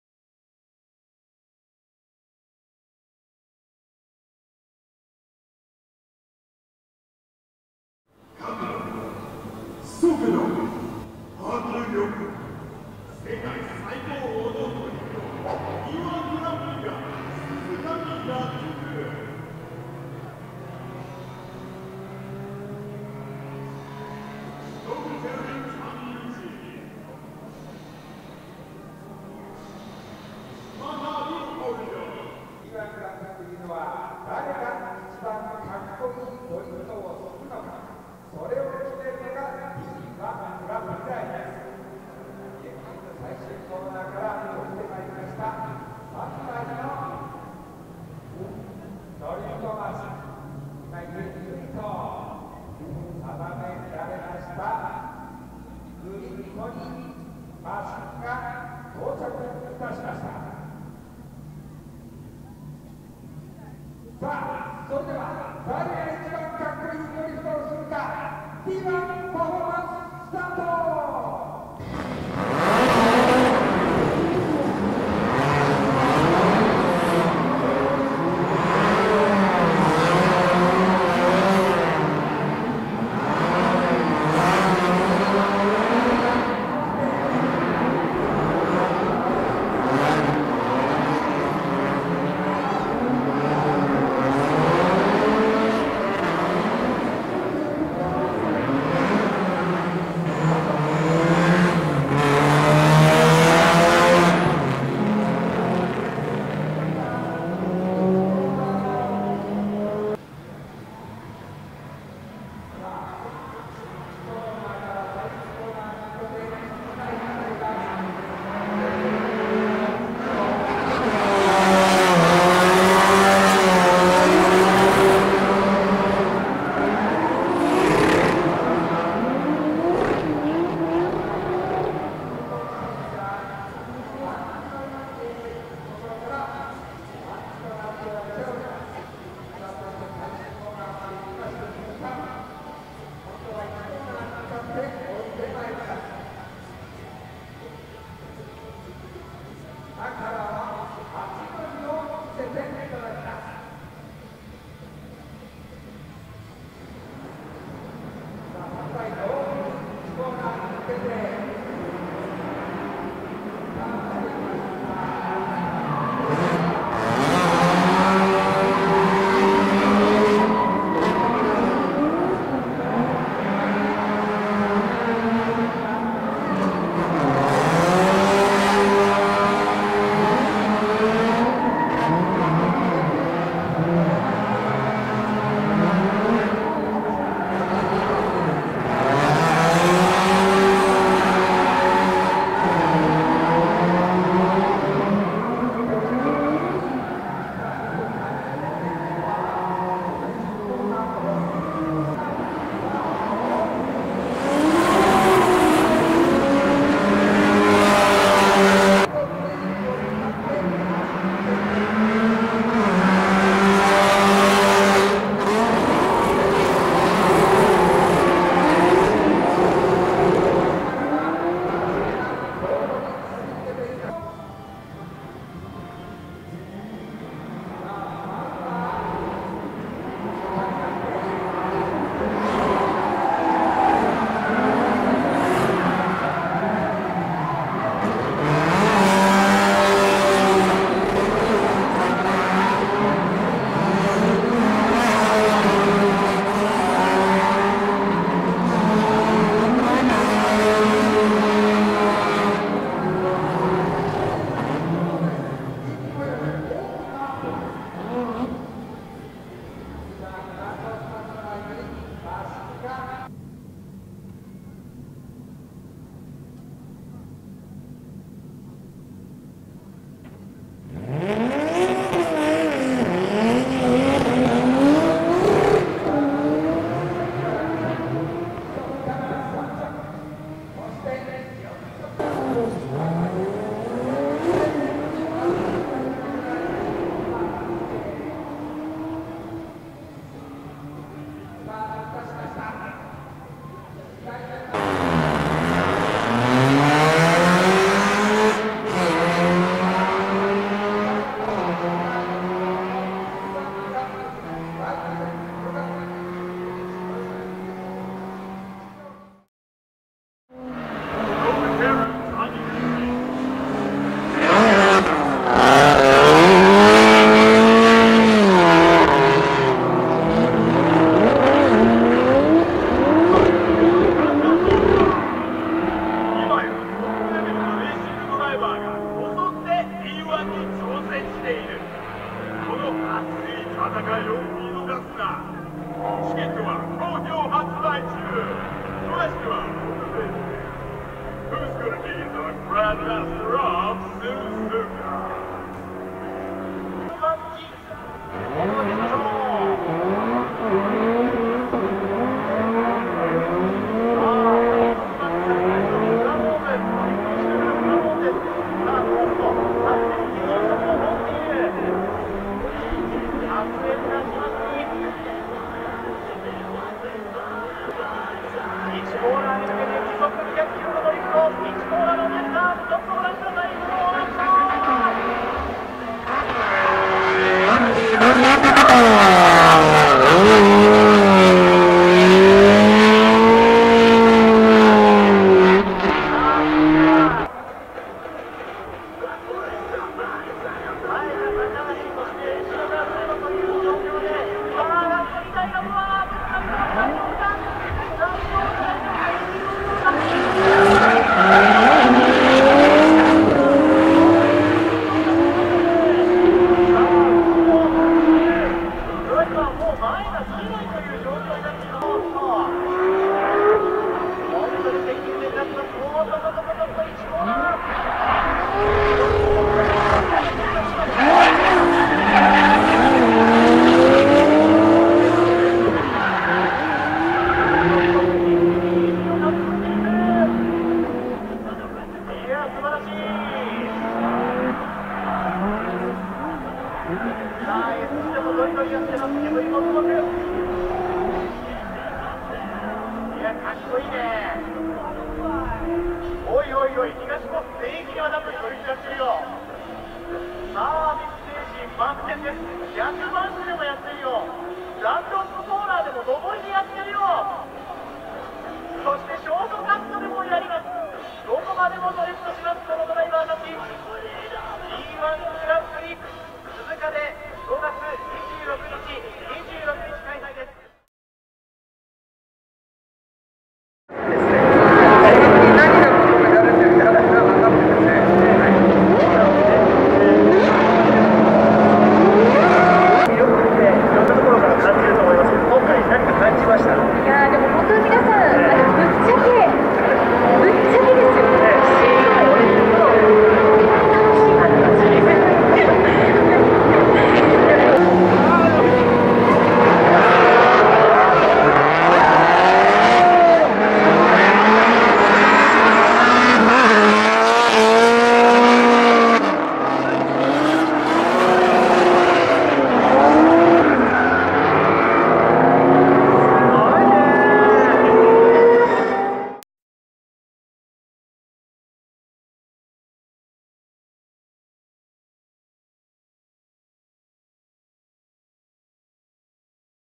[FHD] 時速200キロ高速ドリフト!!! D1 GRAND PRIX 2013 Suzuka プロドリフト選手デモ
D1パフォーマンス

D1グランプリ初年度の2001年から参戦を続けているベテランの高橋邦明と上野高広、そして初代D1グランプリチャンピオンで、SUPER GT GT300でもチャンピオンを獲得している名手、谷口信輝の3名が華麗かつ豪快なパフォーマンスを披露！

日時 3月2日（土） 10：00～10：20
3月3日（日） 9：15～9：30
場所 国際レーシングコース 東コース
トヨタ86／谷口信輝
トヨタマークX／高橋邦明
BMW320i／上野高広

モータースポーツファン感謝デー